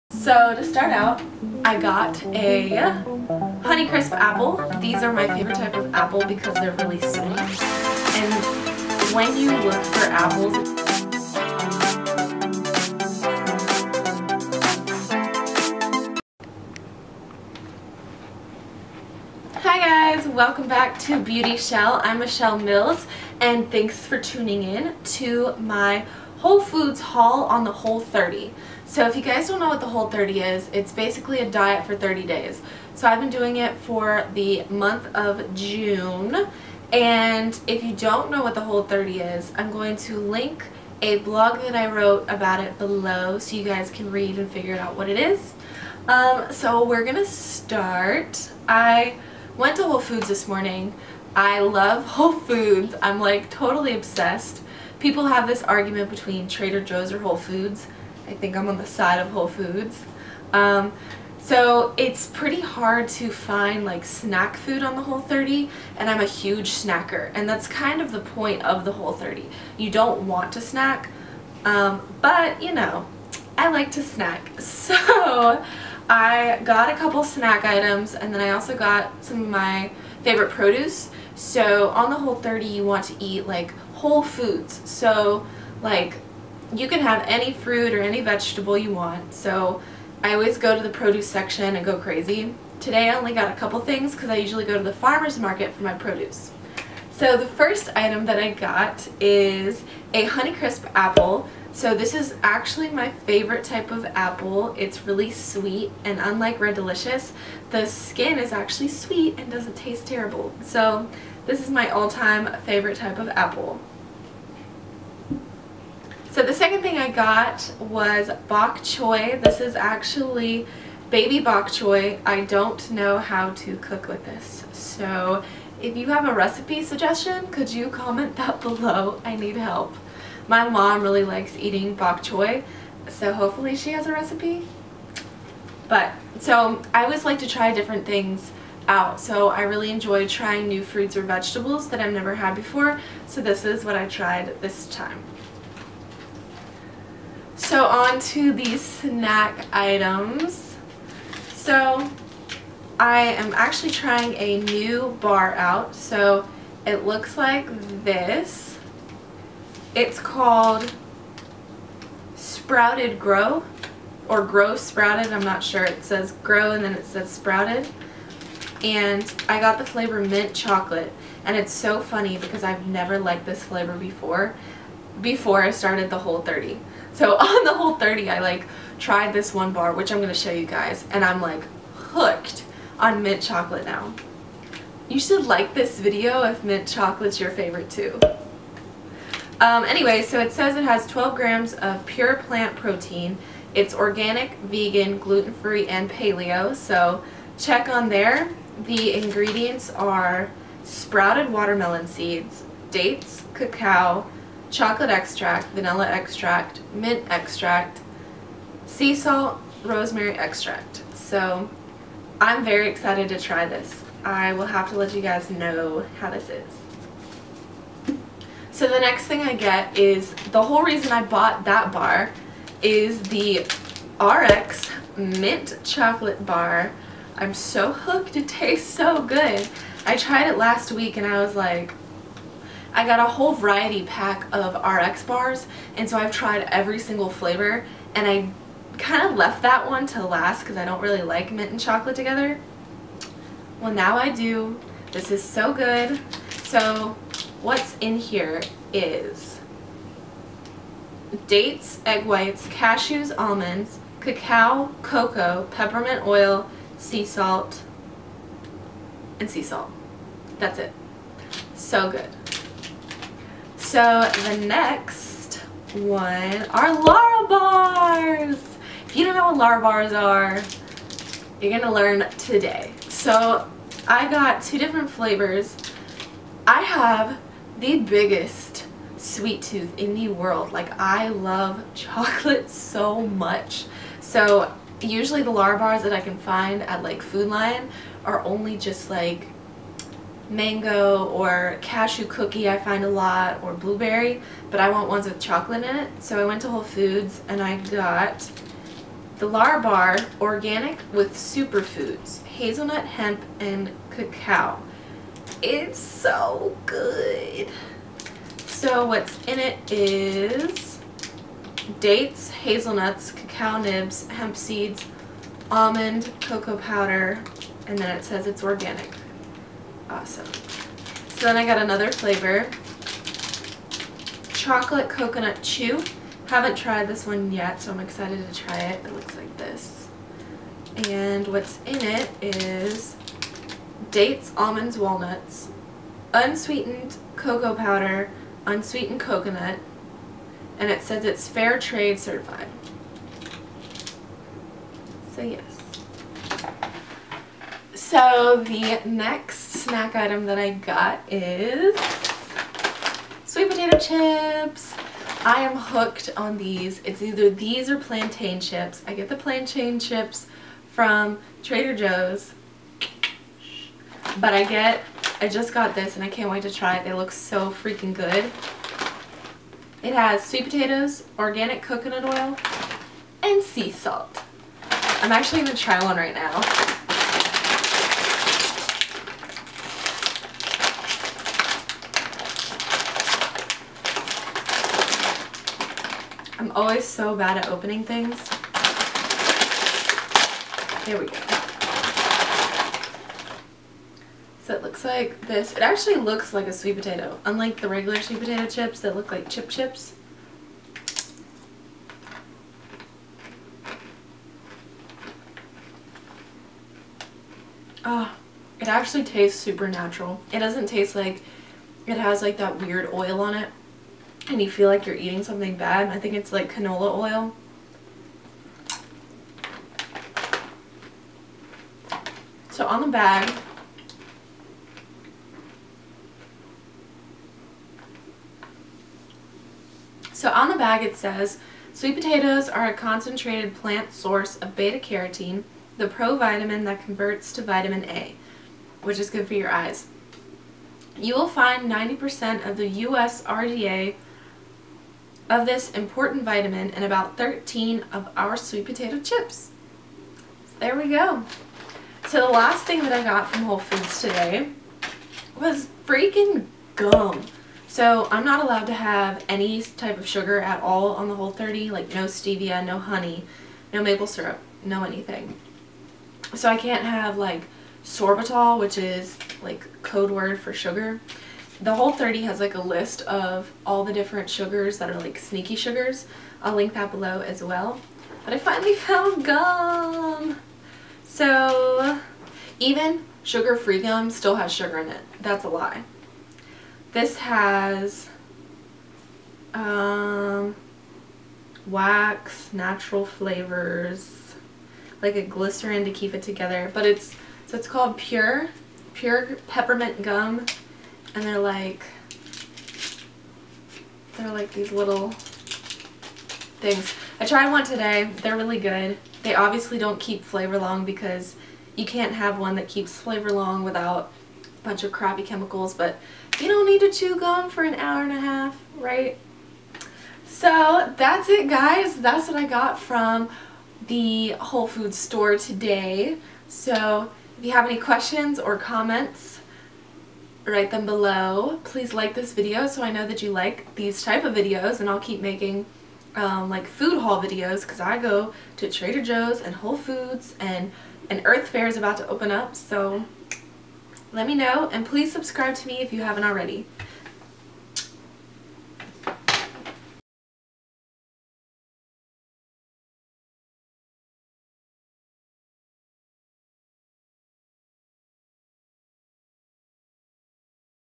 Grocery Haul | What I Buy At WHOLE FOODS
I am obsessed with health food stores and decided to share with you all things I buy from there. I filmed this in June while I was on the Whole 30 so if you are thinking about trying that diet and want snack ideas, you came to the right place. Also popcorn helps :)